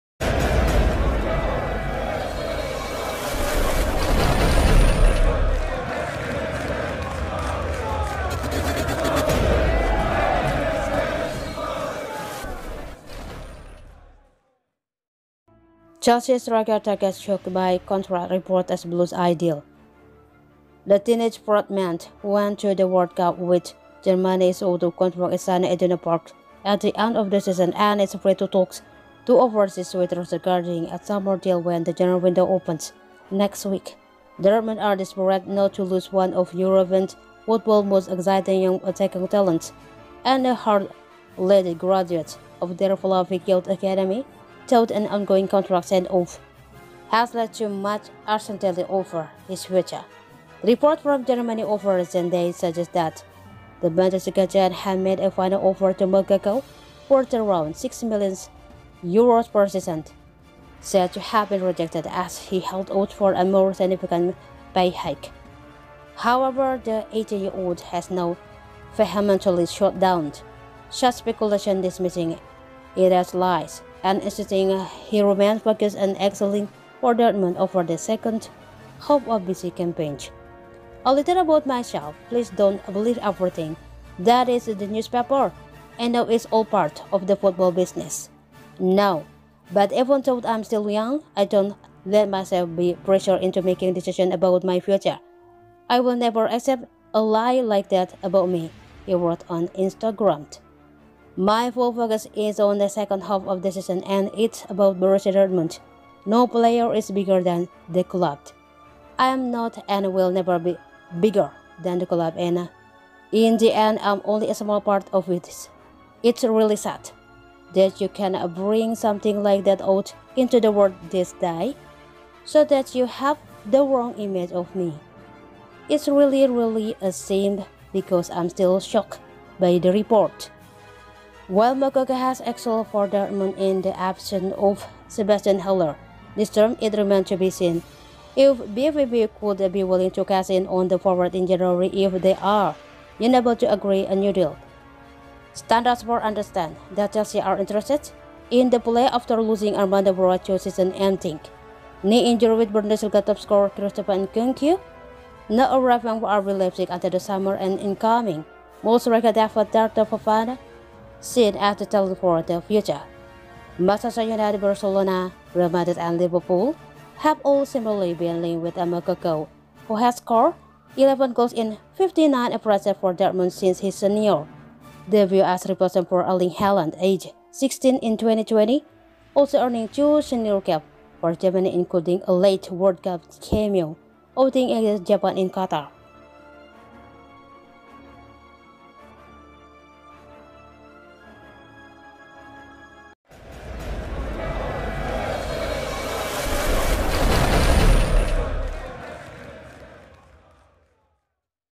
"SHOCK SIGNING": Chelsea striker target ‘shocked’ by contract reports as Blues eye deal
Chelsea striker target ‘shocked’ by contract reports as Blues eye deal